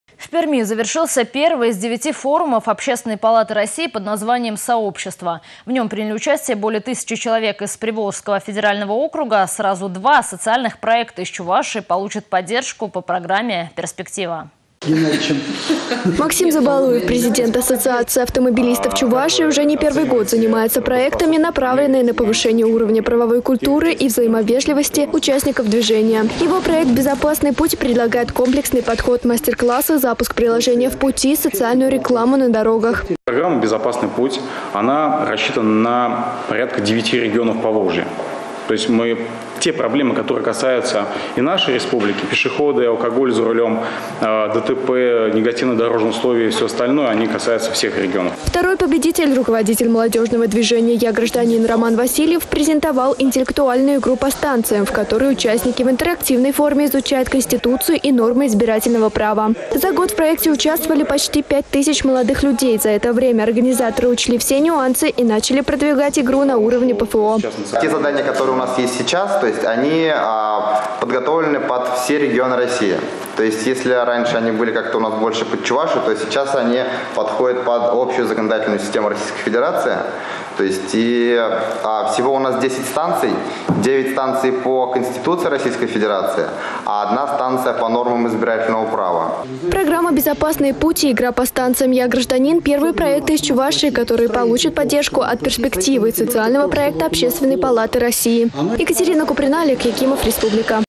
Два социальных проекта из Чувашии получат поддержку по программе «Перспектива».
В Перми завершился первый из девяти форумов  Общественной палаты России под названием «Сообщество». В нем приняли участие более тысячи человек из Приволжского федерального округа.